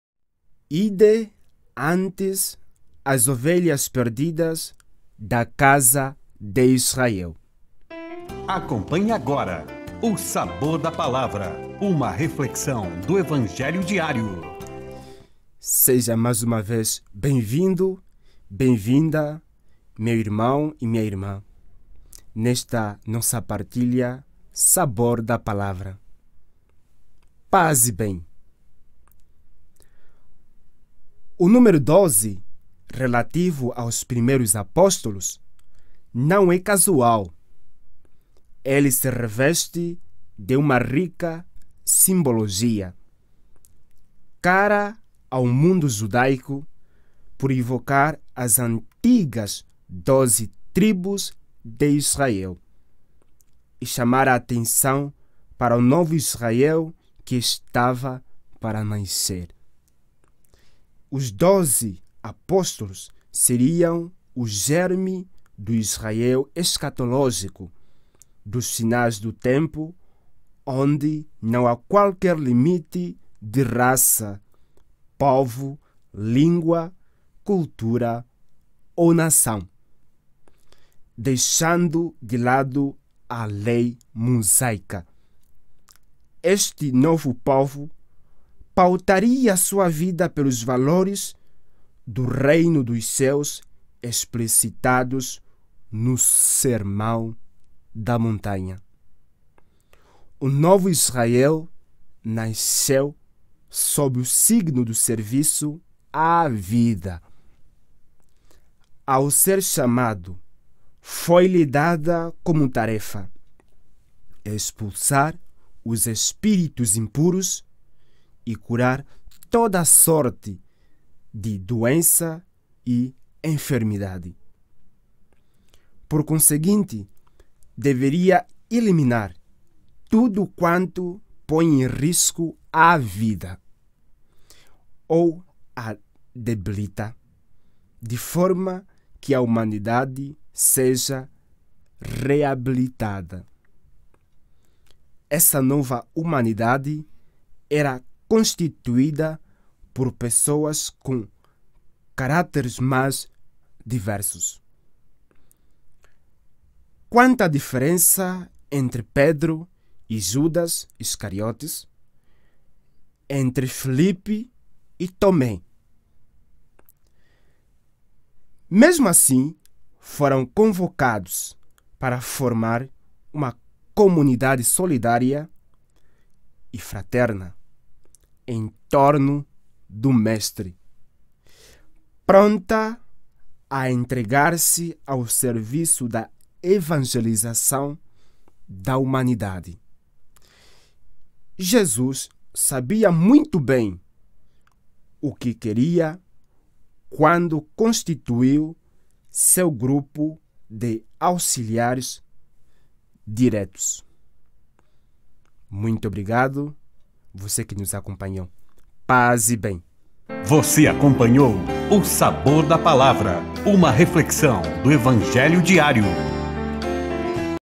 O Sabor da Palavra | Reflexão Diária do Evangelho | 06/07/2022
Acompanhe a Reflexão do Evangelho do Dia (Mt 10,1-7)

“Ide, antes, às ovelhas perdidas da casa de Israel!”

O Sabor da Palavra é preparado pelos Noviços e Frades Estudantes da Província Franciscana da Imaculada Conceição juntamente com a Rádio Construtiva, em Campo Largo - PR.